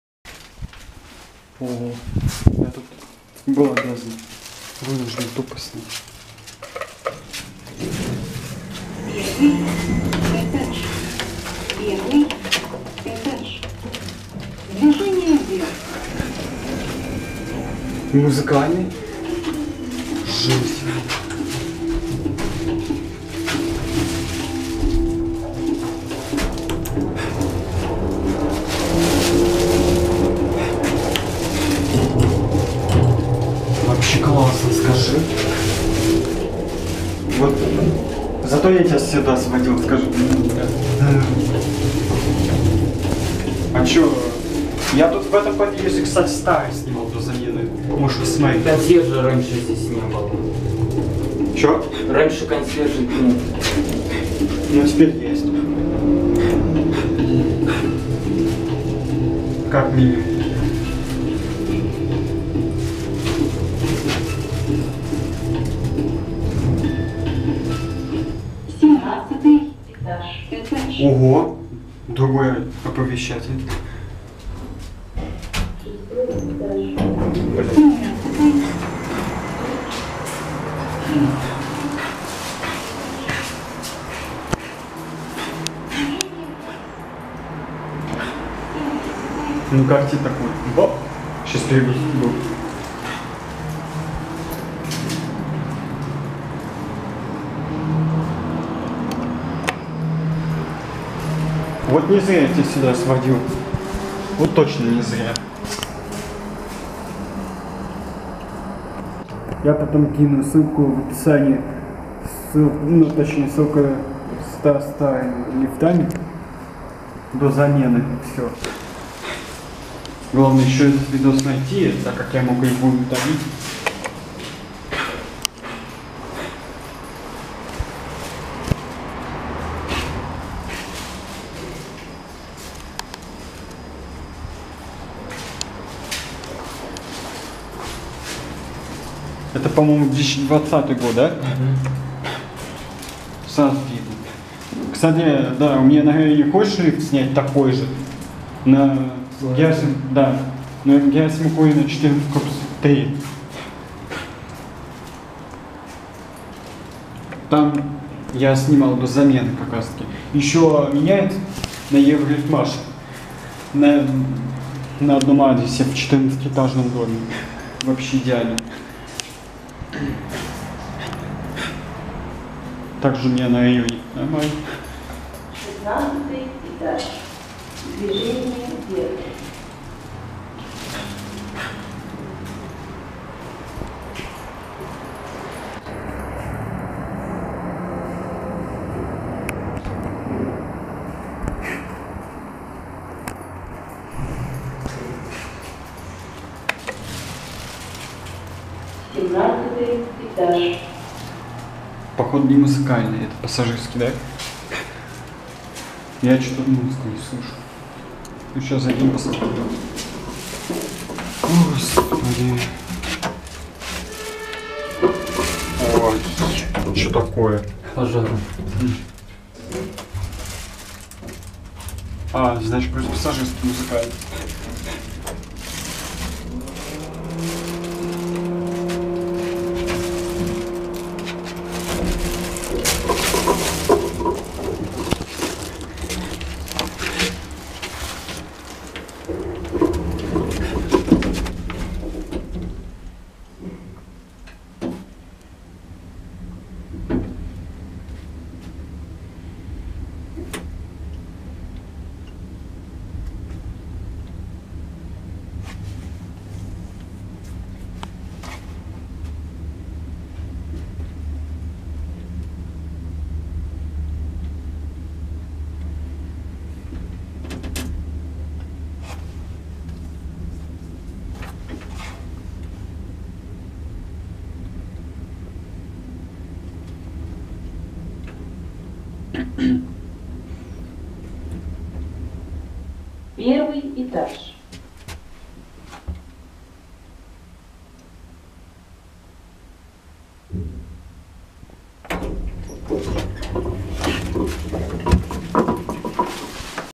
ЛИФТЫ КМЗ СИРИУС-(2020 Г.в) Q=400/kg Q=630/kg ( Грузопасс Музыкальный) V=1m/s Г, Москва Россия
Г. Москва Профсоюзная улица, 124
Этот Дом Занятый Консьержкой
В Видео Обзоре Участвовал Данил Брант

Марка Лифтов: КМз

Модель: Сириус

Скорость V=1m/s